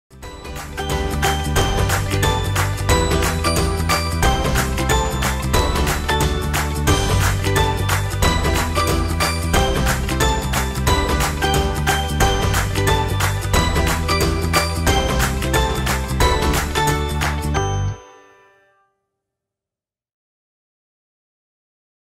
Kamibot Pi Activity: Maze
Kamibot Pi is a coding robot that teaches your child to code through a multitude of activities. Many of our activities require maps and dress up Kamibot Pi using papercraft costumes.